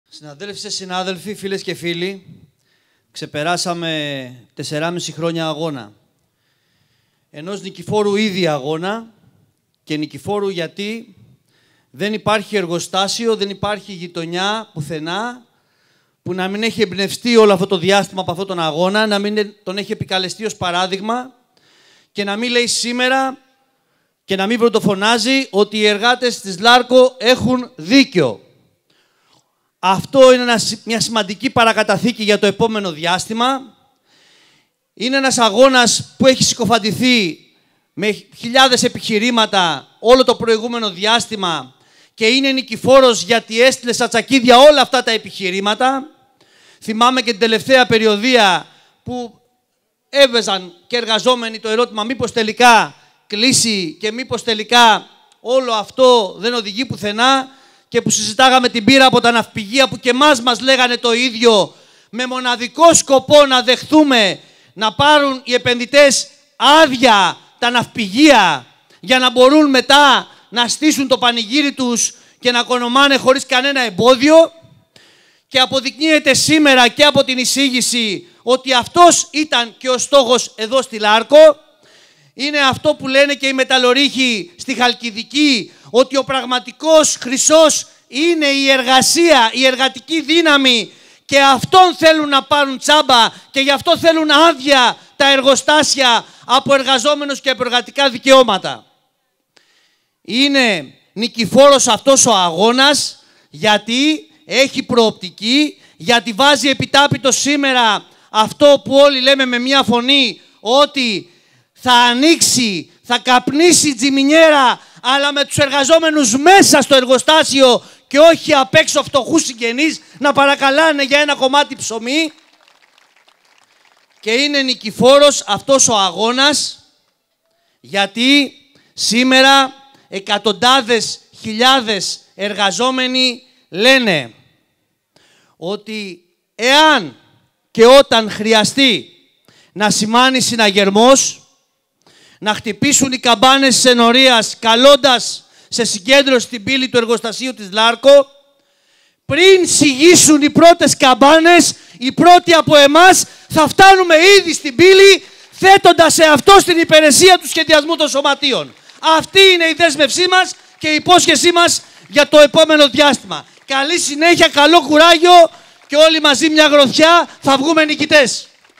Ο Σ. Πουλικόγιαννης στη σύσκεψη φορέων στη ΛΑΡΚΟ
Οι εργαζόμενοι της *ΛΑΡΚΟ,* οι οικογένειες τους δεν είναι μόνοι. Έχουν δίπλα τους τα αδέλφια τους εργάτες, τους βιοπαλαιστές και τα λαϊκά νοικοκυριά ολόκληρων περιοχών. Μια γροθιά, ενωμένοι και αποφασισμένοι να κλιμακώσουν και να γενικεύσουν αυτόν τον αλύγιστο αγώνα ενάντια στους εγκληματικούς σχεδιασμούς και την αντιλαϊκή πολιτική της κυβέρνησης, που διαλύει τη ΛΑΡΚΟ, ξεκληρίζει την αγροτιά και τους αυτοαπασχολούμενους, ερημώνει τα χωριά και τον τόπο τους. Αυτό είναι το ξεκάθαρο και αποφασιστικό μήνυμα αγώνα που έστειλε η μεγάλη σύσκεψη στον οικισμό του εργοστασίου της ΛΑΡΚΟ στη Λάρυμνα. Μία σύσκεψη που πήρε χαρακτηριστικά μαζικής και δυναμικής κινητοποίησης, ενός ακόμη μεγάλου αγωνιστικού σταθμού που παίρνει τη σκυτάλη από τις συγκλονιστικές κινητοποιήσεις του προηγούμενου διαστήματος, προετοιμάζοντας παράλληλα τη διεύρυνση και συνέχιση του αγώνα με νέες δυναμικές και πολύμορφες κινητοποιήσεις, για να καπνίσει ξανά η τσιμινιέρα της ΛΑΡΚΟ, για να ζήσουν αξιοπρέπεια στον τόπο τους οι βιοπαλαιστές αγρότες, κτηνοτρόφοι και επαγγελματίες, ο λαός της περιοχής.